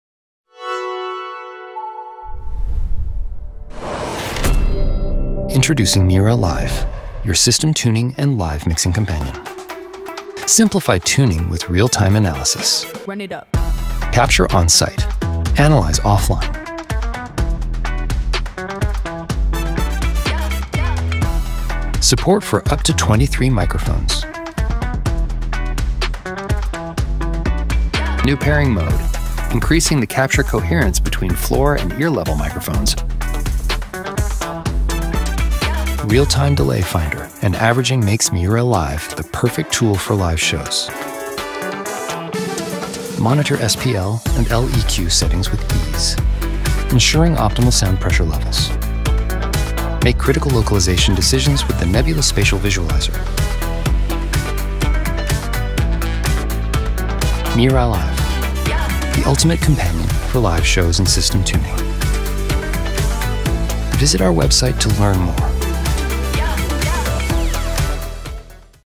MiRA Live - Quick Look | Audio Analysis & Metering Software from FLUX::
Master the technical fundamentals of audio system calibration and live mix with confidence in a reactive real-time environment with MiRA Live which includes a suite of dependable tools, including Transfer Function readings, Magnitude, Phase & Coherence traces, and live impulse response, or real-time finder functions for delay computation with the ability to recompute captures.

* CAPTURE – ANALYZE – SIMPLIFY YOUR SYSTEM TUNING SESSION - MiRA Live features advanced session management for system measure captures, with offline recompute support.

* ADVANCED MICROPHONE CAPTURES - Support for up to 23 measurement microphones, featuring a real-time delay finder and averaging capabilities. New pairing mode increases the capture coherence between floor and ear level microphones.

* LIVE SHOW LAYOUT - Choose your SPL/Leq microphone and define your settings on a Live show layout that includes three independent SPL/Leq measurement settings.

* NEBULA SPATIAL VISUALIZER - Make critical localization decisions for your mix using MiRA Live and the unique FLUX:: Nebula spatial spectrogram. Monitor the output of SPAT Revolution or your favorite immersive processor output.